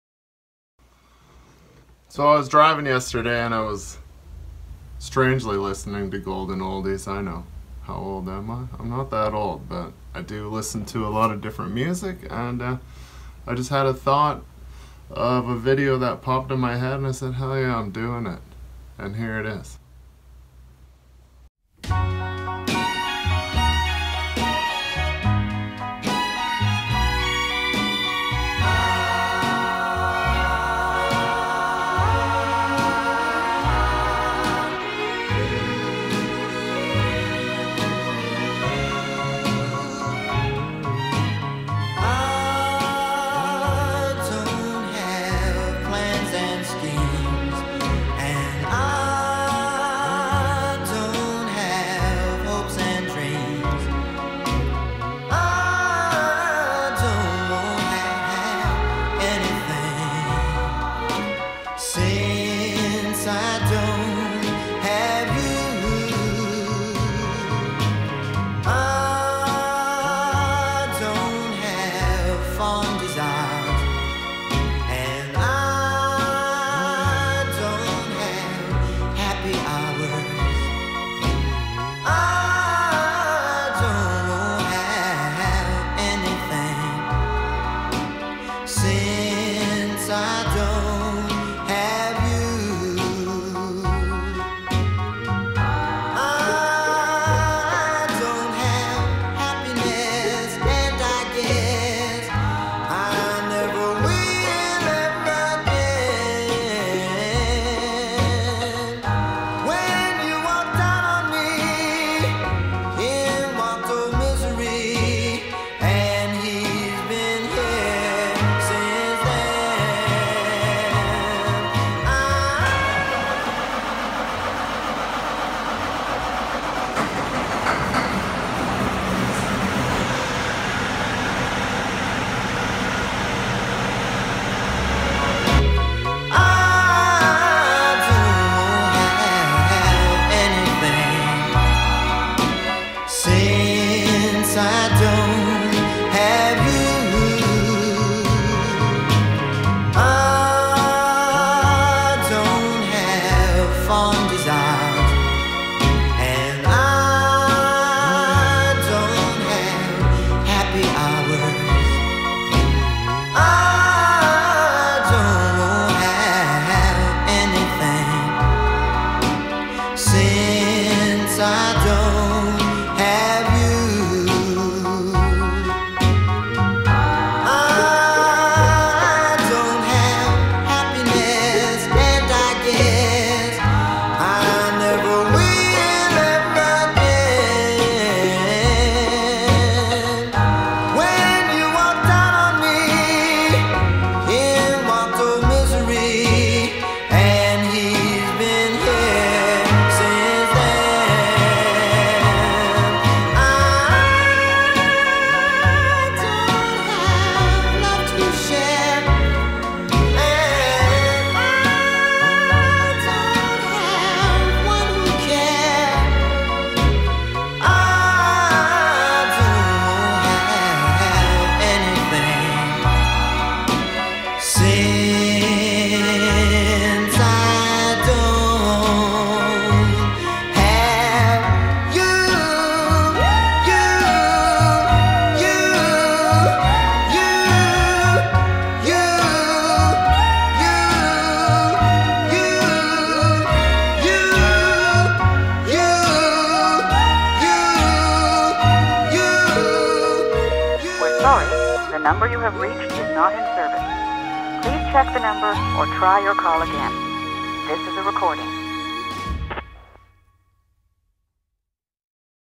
HOMAGE TO T185 A PICTURE FILM
Some of the greater moments from roadtrain days